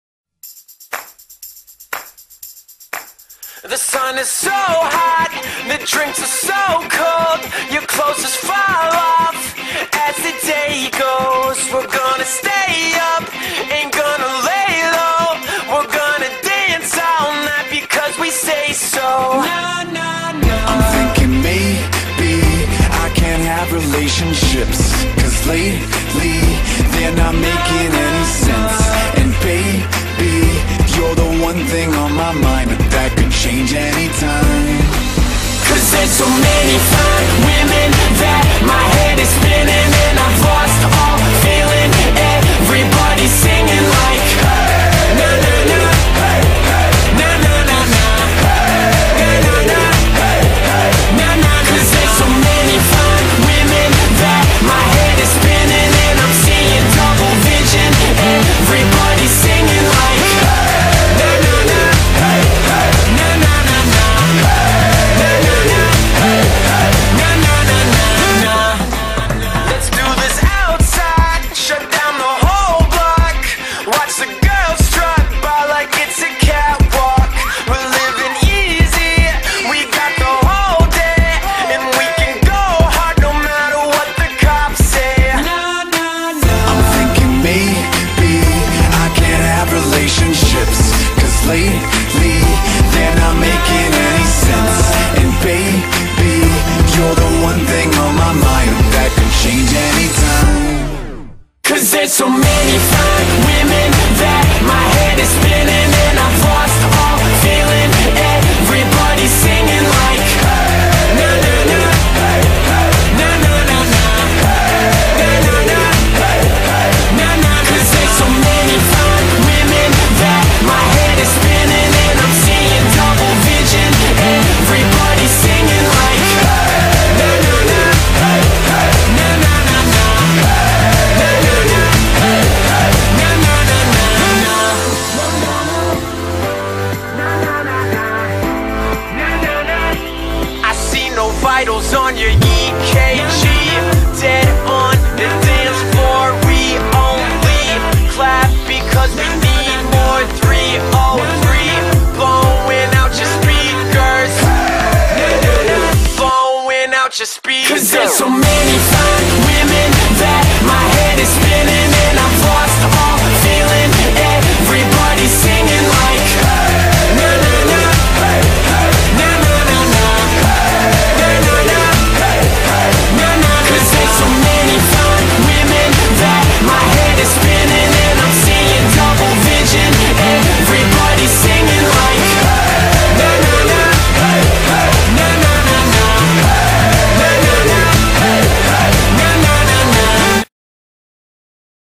Bill Kaulitz has double vision =)
bi watched a video of bill kaulitz saying he hadnt not had a relationship for 5 years etc :( poor bill :'( so anyway i thought this was cute with him :)